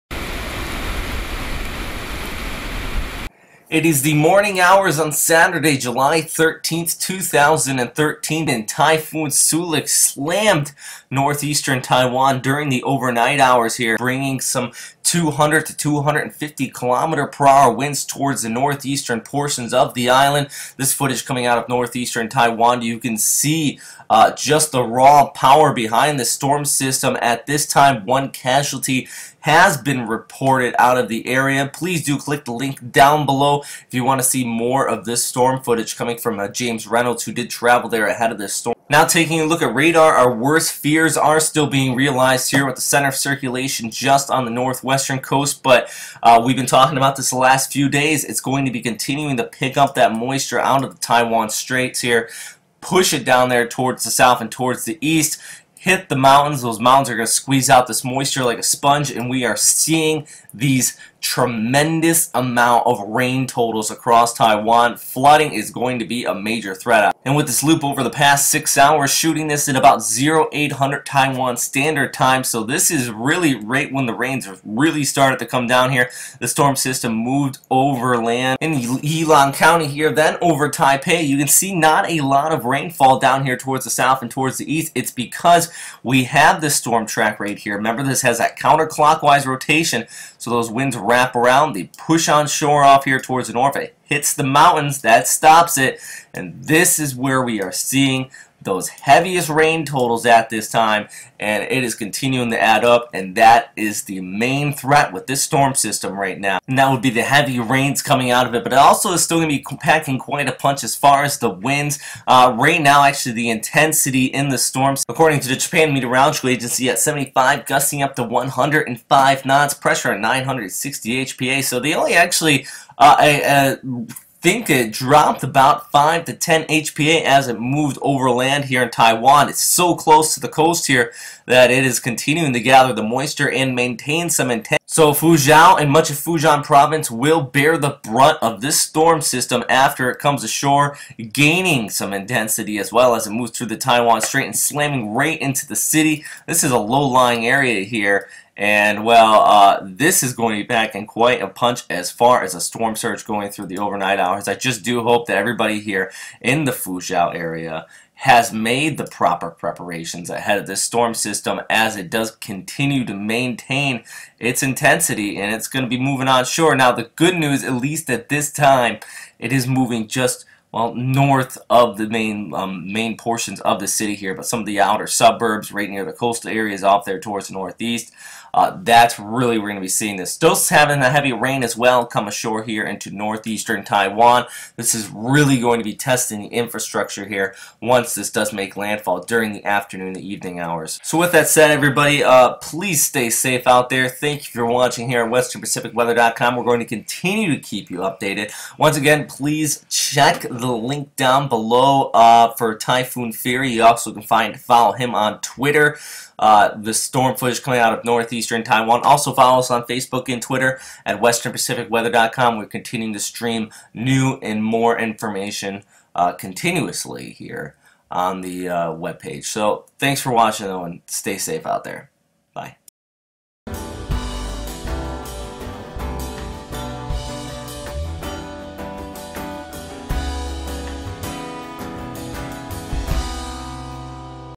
Typhoon Soulik Becomes Deadly, Storm Footage and Forecast
Typhoon Soulik made landfall in North Eastern Taiwan during the overnight hours on July 12th through 13th packing winds over 200kph as it came ashore. As of writing this update one casualty has been reported by local media due to the storm.  As the storm continues to cruise west towards Fuzhou China rains will continue to batter much of Central Taiwan. The image to the right shows just how much has come down today. Some totals climbing over the 500mm mark in just a few short hours.  It is the areas in the reds and purples on this map that are at greatest risk for flooding and landslides as moisture continues to stream ashore on to the island throughout the day. Our video update above talks about this and also the latest information coming in out of Taiwan.

Typhoon Storm Footage